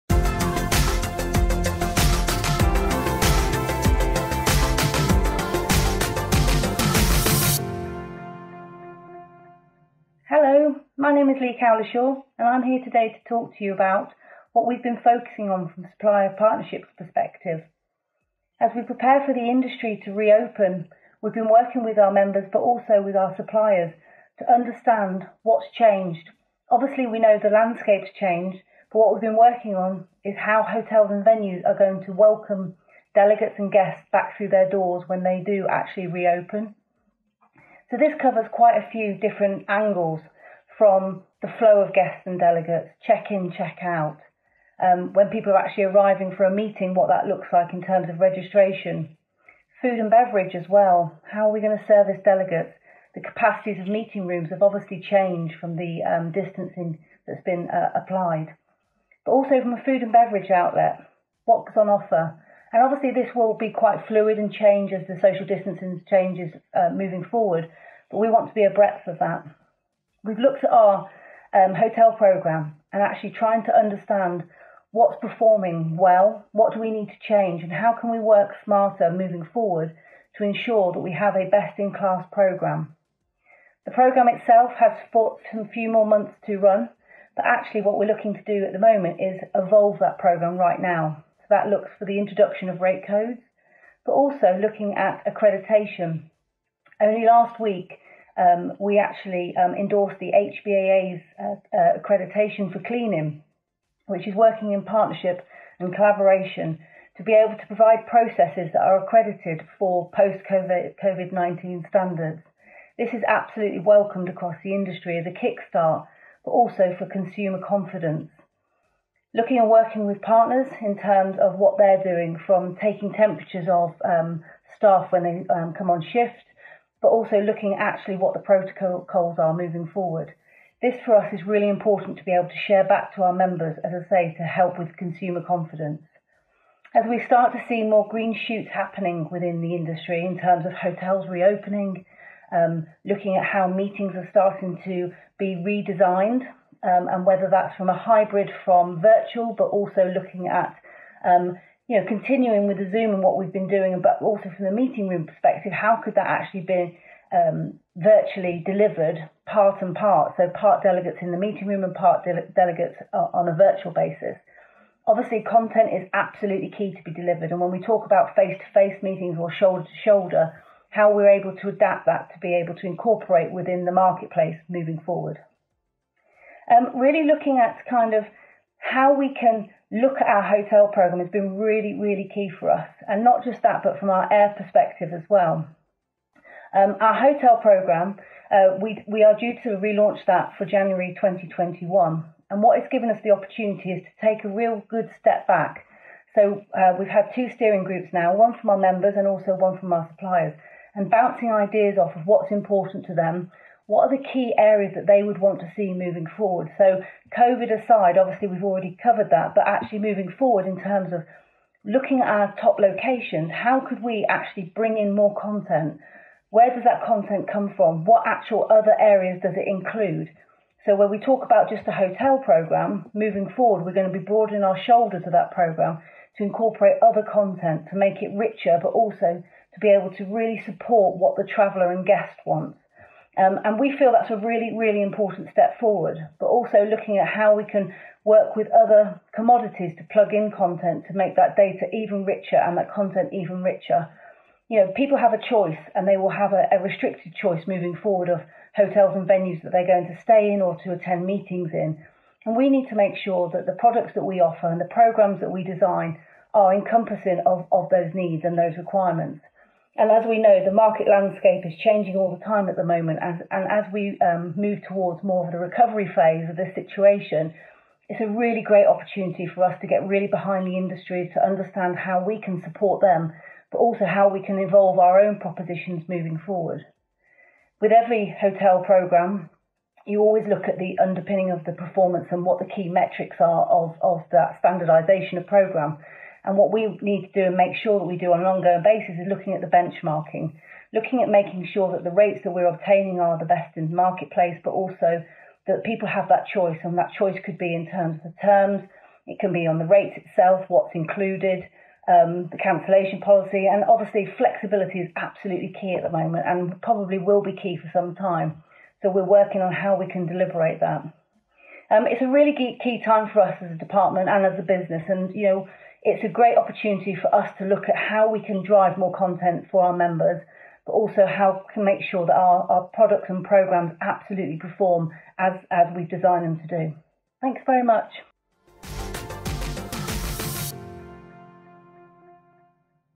Advantage Update - 28 May 2020
Advantage Update Video on 28 May 2020

In today's video update, we have Leigh Cowlishaw, Global Supplier Director at Advantage talking about what we have been focusing on from a supplier partnerships perspective on the corporate travel side.